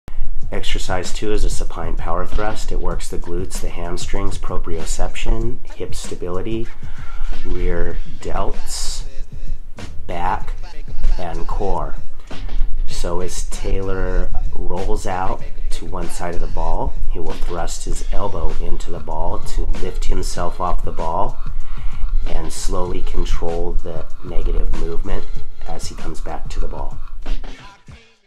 Exercise Mini Video 2
In this video, Taylor shows you a Supine Power Thrust which works  your glutes, hamstrings, back and core.  Give it a shot!